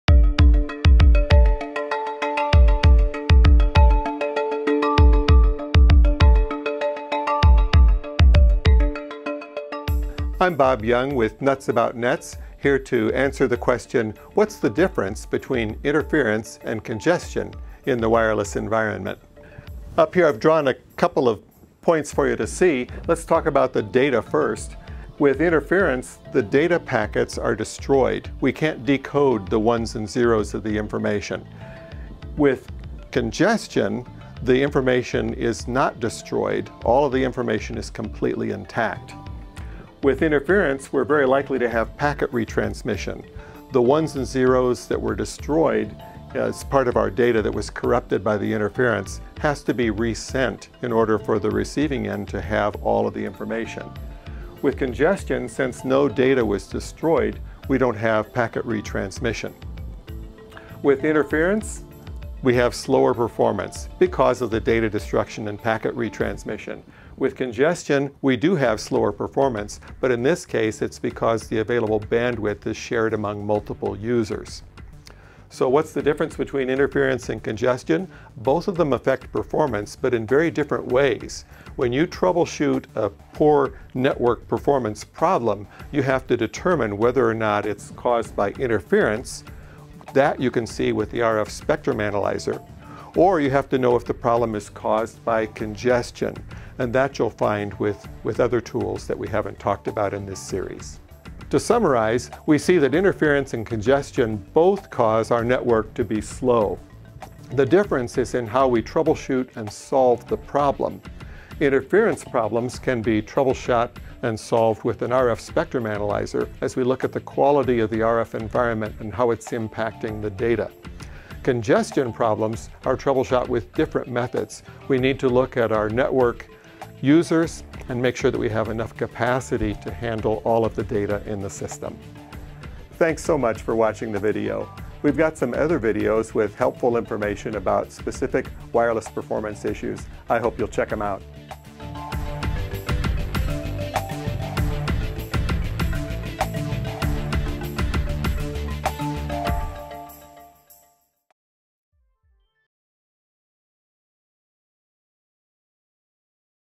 WiFi Interference vs WiFi Congestion with Bob Young from FIFO Networks by Nuts About Nets
Bob Young with Nuts About Nets describes the difference between WiFi interference and WiFi congestion. With interference, data packets are destroyed and retransmission is required. With congestion, the available bandwidth is inadequate for the number of users and the amount of data. Both issues result in compromised wireless performance, but the troubleshooting method and solution for each issue are very different.

Video Transcript:
I'm Bob Young with Nuts About Nets here to answer the question, "What's the difference between interference and congestion in the wireless environment?"

Up here I've drawn a couple of points for you to see. Let's talk about the data first. With interference, the data packets are destroyed. We can't decode the ones and zeros of the information. With congestion, the information is not destroyed, all of the information is completely in tact.

With interference, we're very likely to have packet retransmission. The ones and zeros that were destroyed as part of our data that was corrupted by the interference has to be re-sent in order for the receiving end to have all of the information.

With congestion, since no data was destroyed, we don't have packet retransmission.

With interference, we have slower performance because of the data destruction and packet retransmission. With congestion, we do have slower performance, but in this case it's because the available bandwidth is shared among multiple users.

So what's the difference between interference and congestion? Both of them affect performance, but in very different ways. When you troubleshoot a poor network performance problem, you have to determine whether or not it's caused by inference. That you can see with the RF Spectrum Analyzer. Or you have to know if the problem is caused by congestion. And that you'll find with other tools that we haven't talked about in this series.

To summarize, we see that interference and congestion both cause our network to be slow. The difference is in how we troubleshoot and solve the problem. Interference problems can be troubleshot and solved with an RF Spectrum Analyzer as we look at the quality of the RF environment and how it's impacting the data.

Congestion problems are troubleshot with different methods. We need to look at our network users and make sure that we have enough capacity to handle all of the data in the system.

We've got some other videos with helpful information about specific wireless performance issues. I hope you'll check them out.